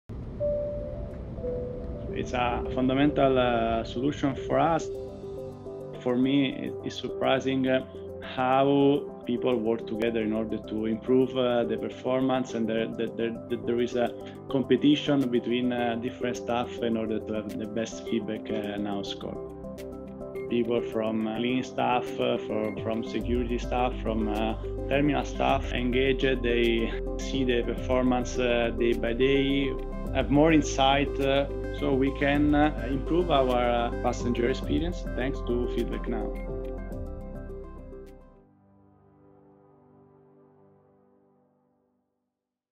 FeedbackNow at the Airport - Improve Passenger Satisfaction
FeedbackNow is used at airports worldwide to increase passenger satisfaction and improve operations.

In this video we learn how FeedbackNow can be gamified to drive staff benchmarks.